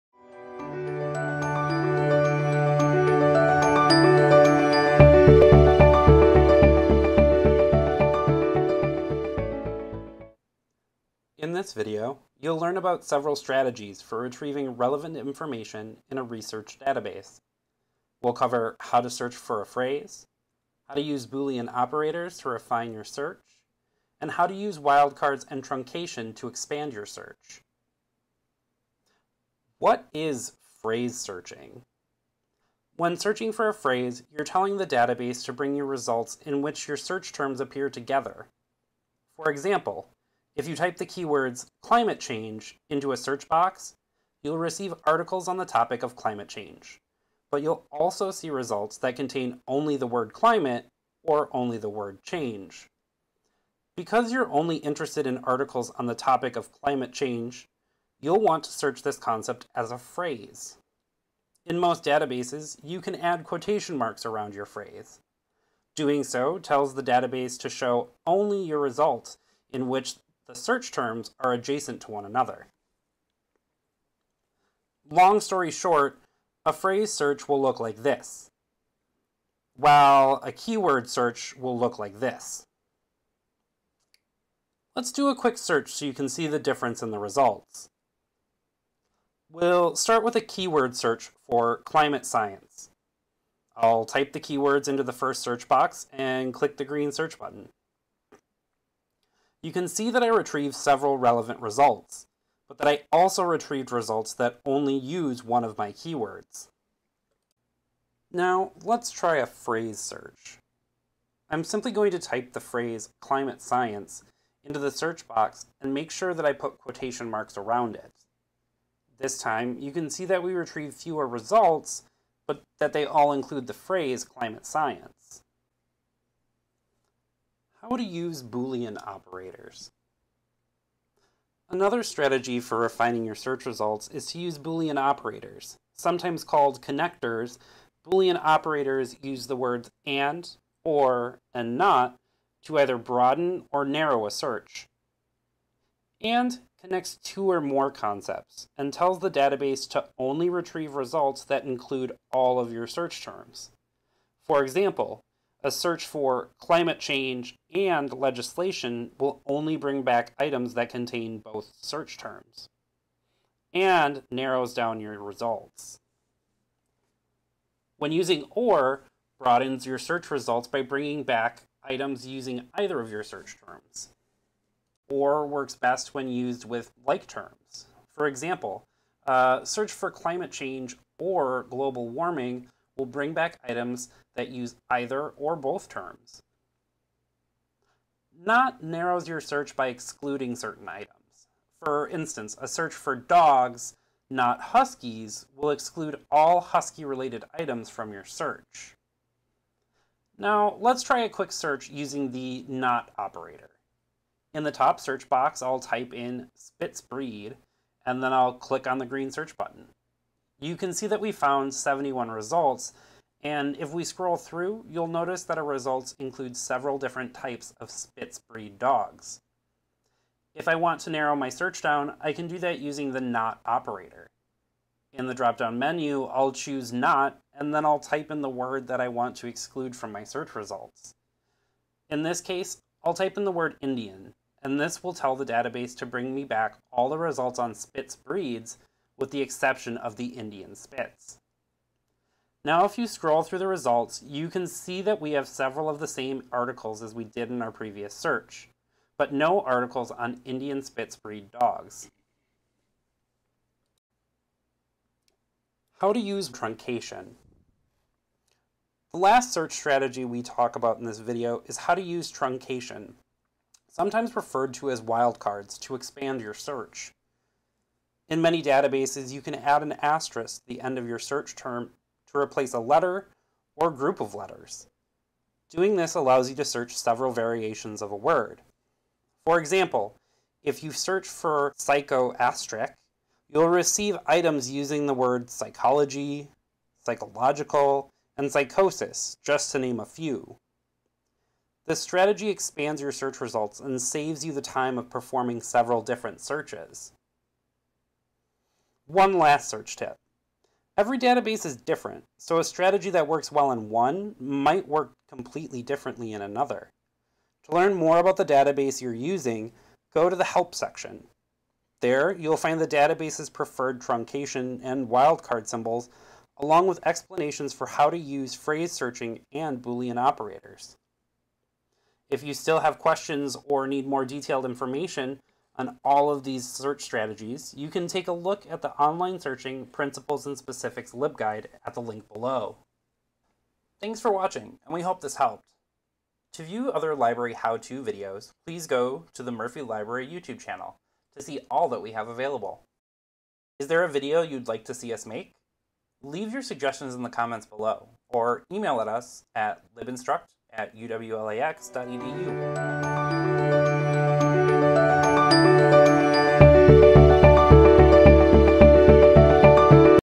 Search tips & tricks
In this video, you’ll learn about several strategies for retrieving relevant information in a research database. We’ll cover how to search for a phrase, how to use Boolean operators to refine your search, and how to use wildcards and truncation to expand your search.